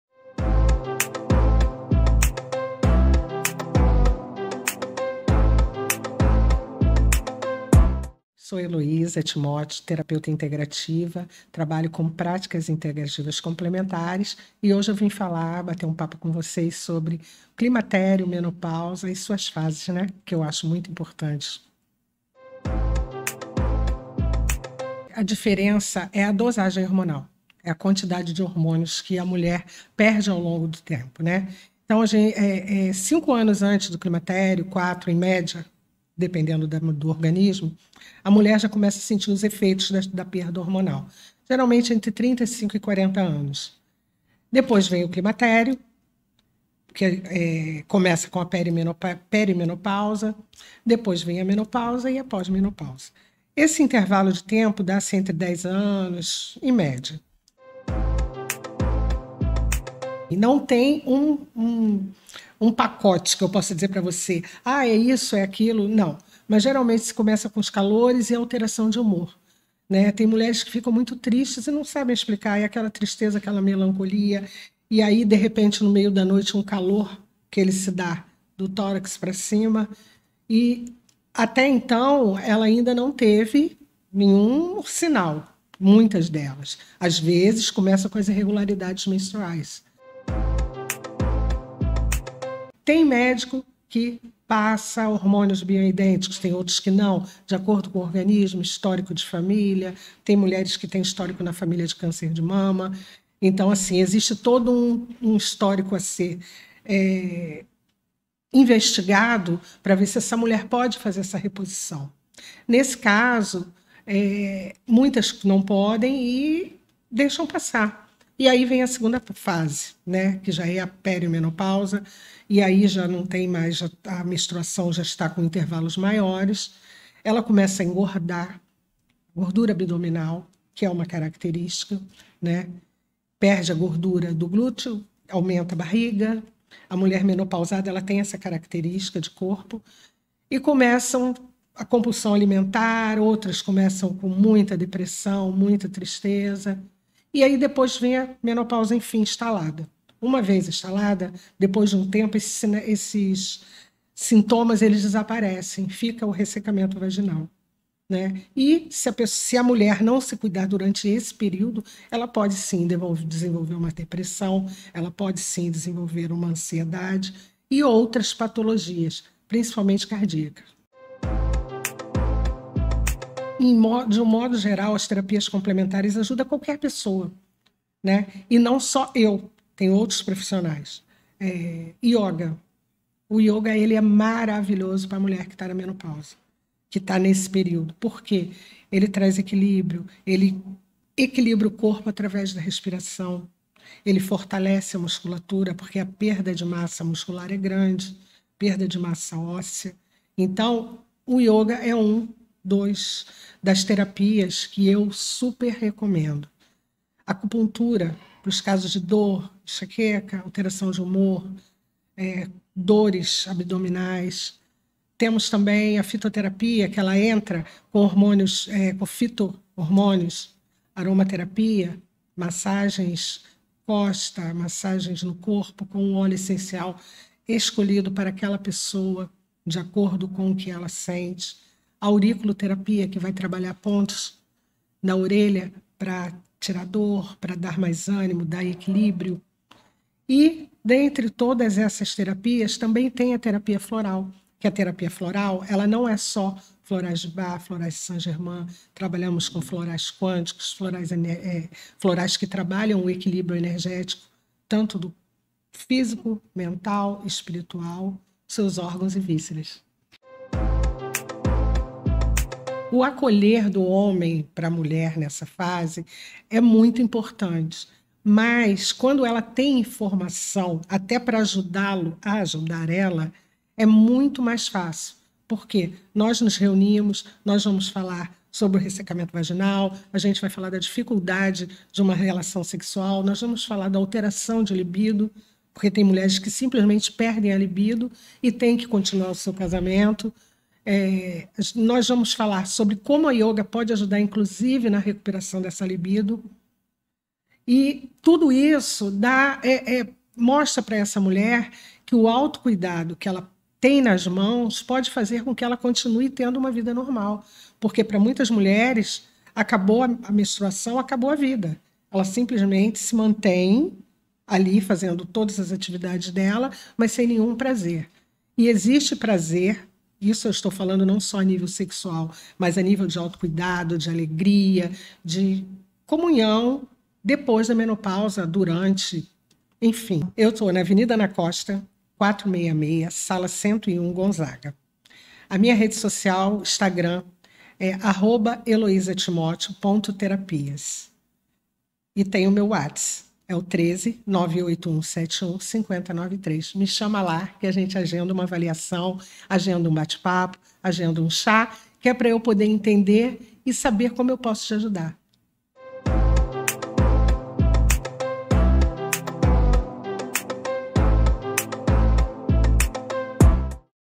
Benefícios das terapias alternativas durante o climatério | Em dia com a saúde
A terapeuta holística Heloisa Timóteo explica quais são as fases do climatério e orienta sobre as terapias alternativas mais indicadas para as mulheres que estão passando pelo período da menopausa.